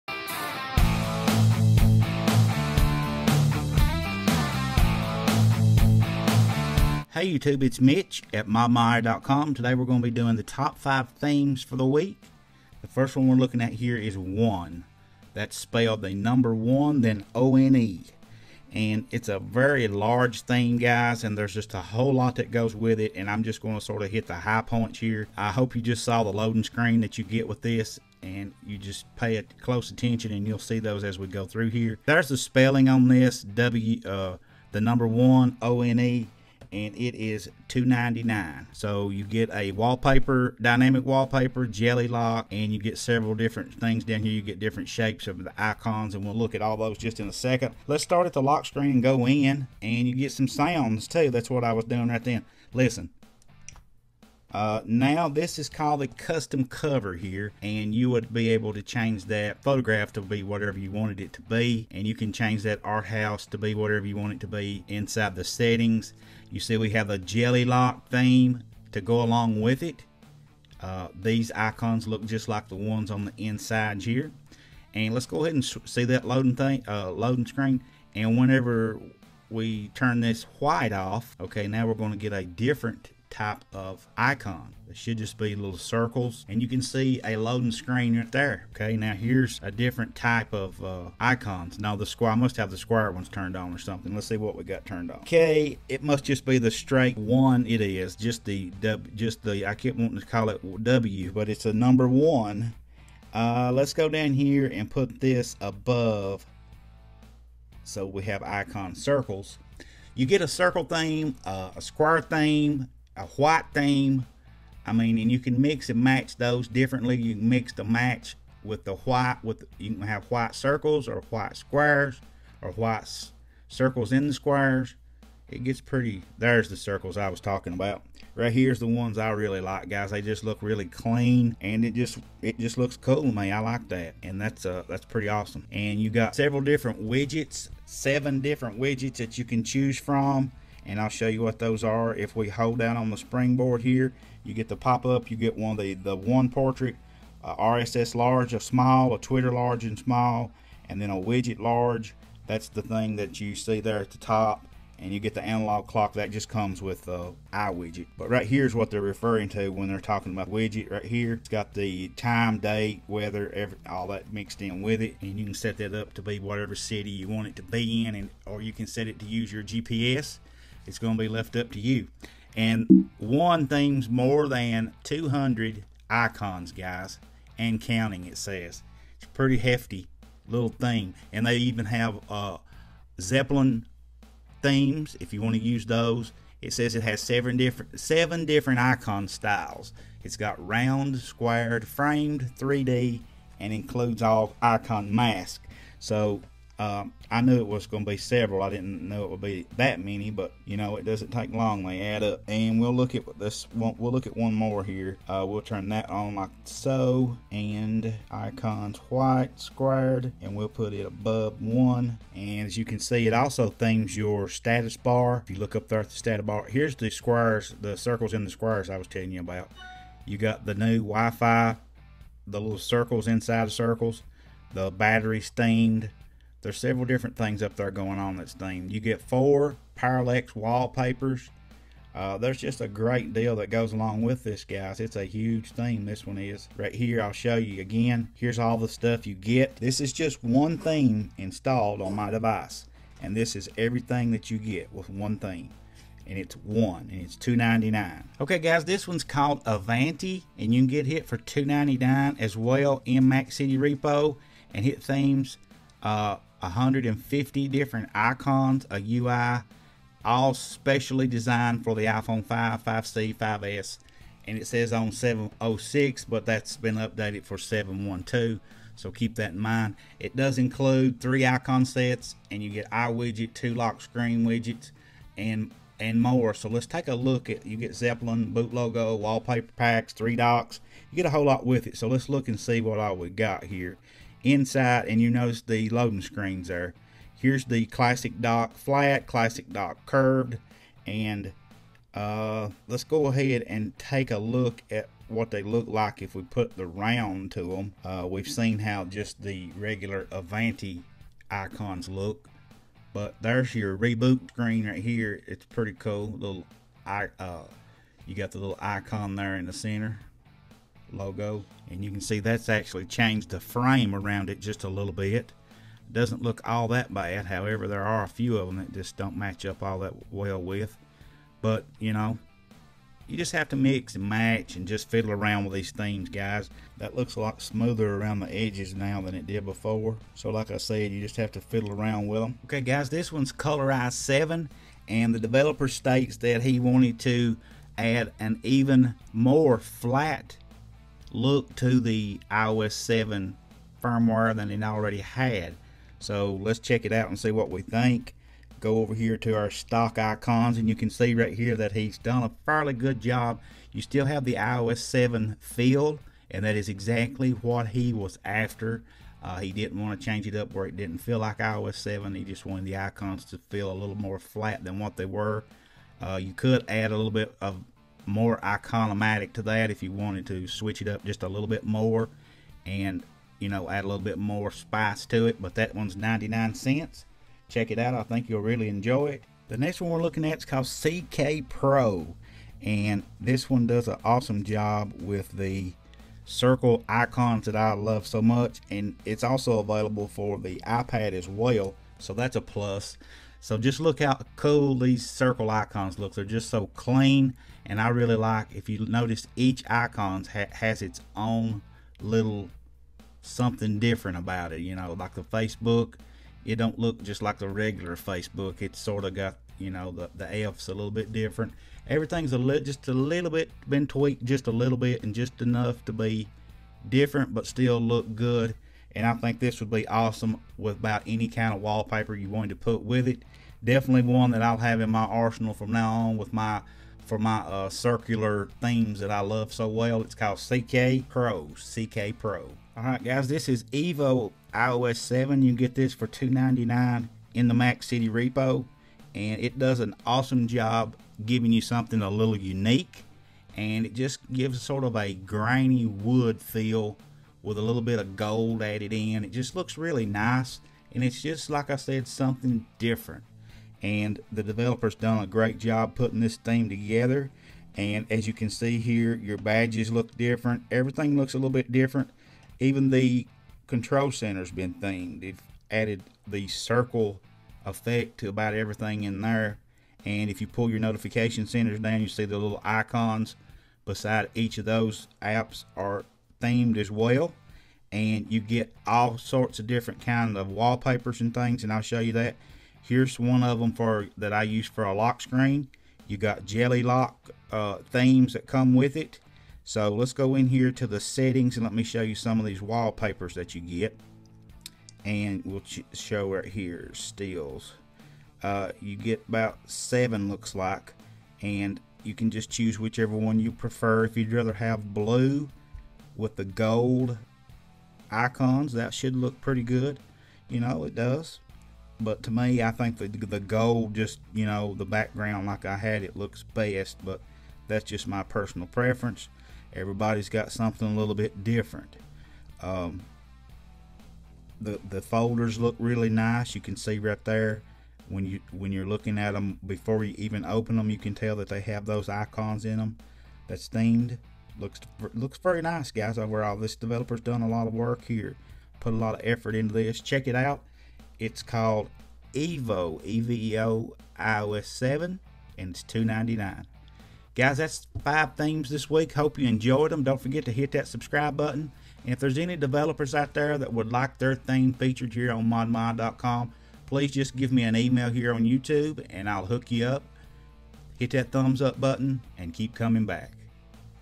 Best Five Winterboard Themes iOS 7.1.2 Pangu Jailbreak July 28, 2014
Best Five Winterboard Themes July 28, 2014
1one  $2.99 ModMyi Reoi
Avanti $2.99  MacCiti Repo
CK Pro Free  ModMyi Repo
Evo iOS 7 $2.99  MacCiti Repo
Colorize7  $0.99  MacCiti Repo

SOCIAL US